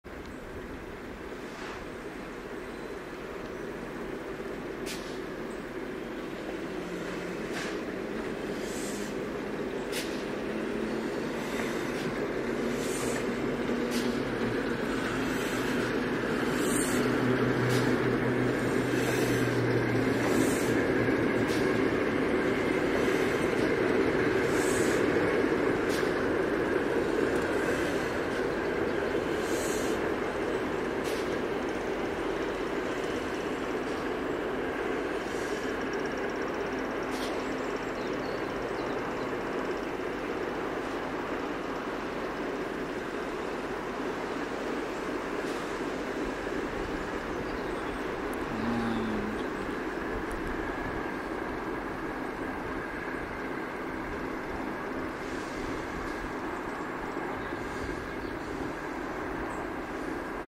SVR DDay 2013 1 The humble 08 puts away the steamer
Our first video from this set covers the humble 08 moving steamers around and here 5047 gets shunted back to the shed, cylinders whooshing as she does so. (C) RCT 2013